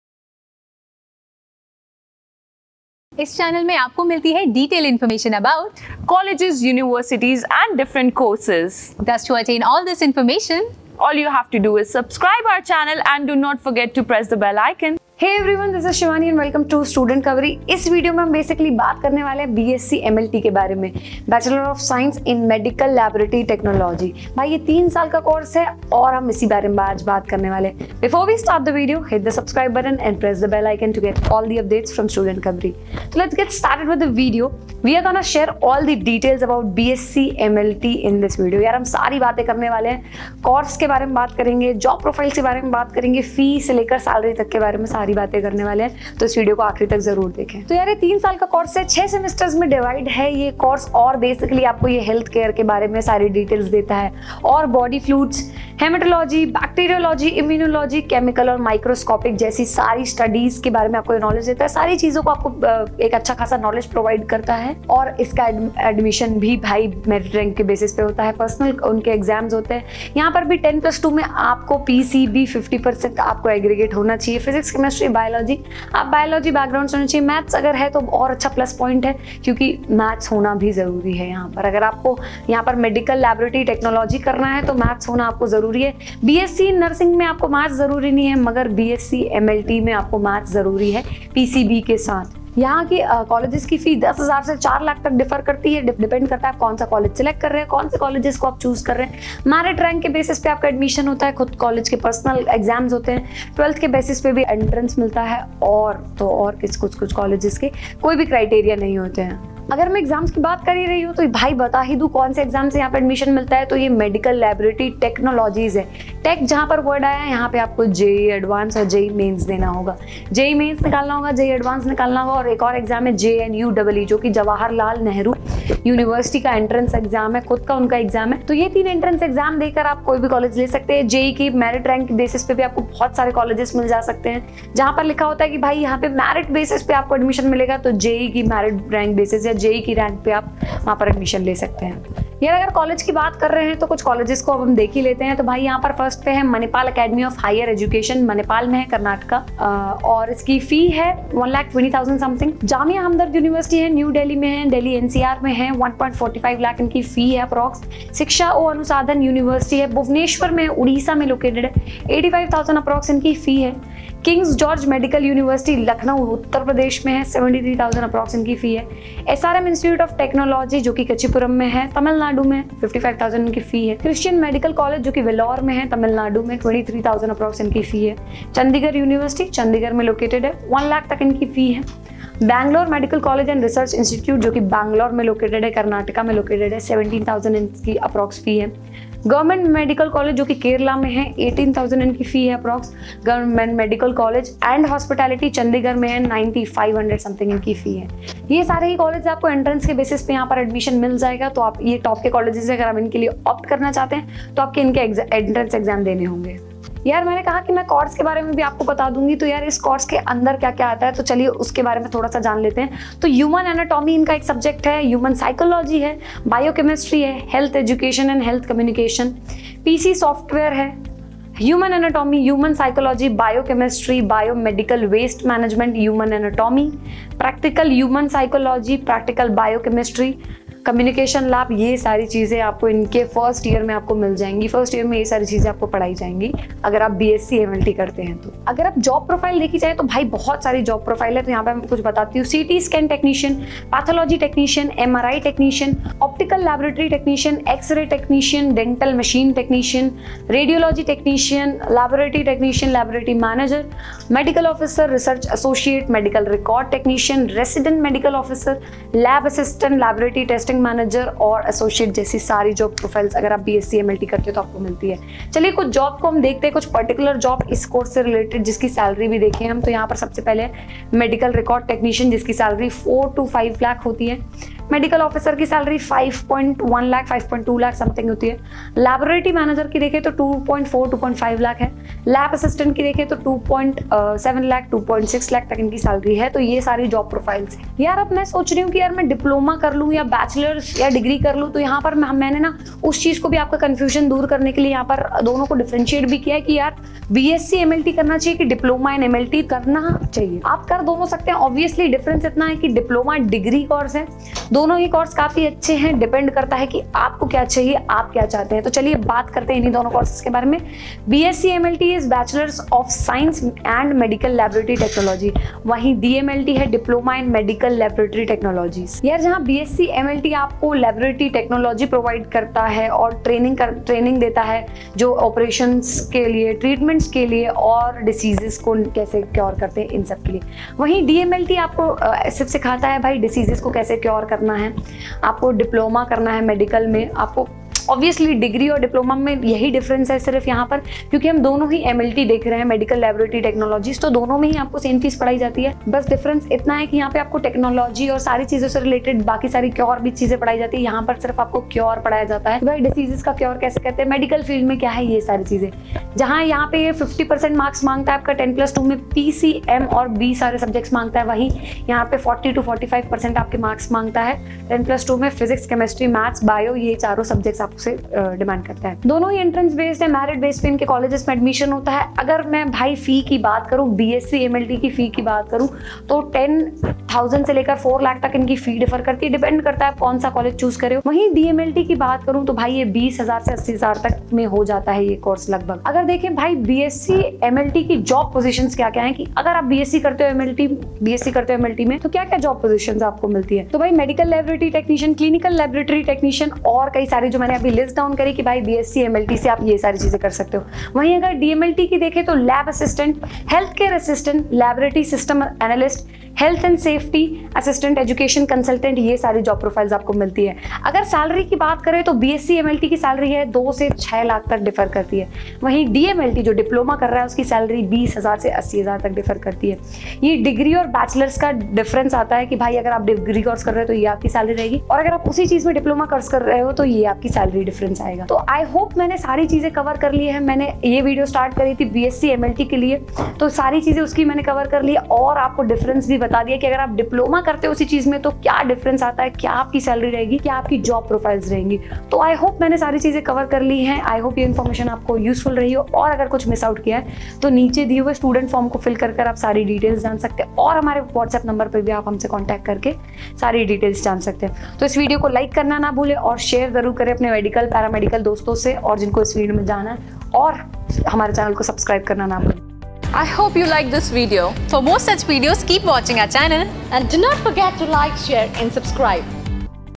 B.Sc in Medical Laboratory Technology | Admission, Eligibility, Exam, Top Colleges, Scope, Salary
Hello Everyone
In this video, you will know: B.Sc in Medical Laboratory Technology

Get detailed information about MBA / PGDM colleges, courses.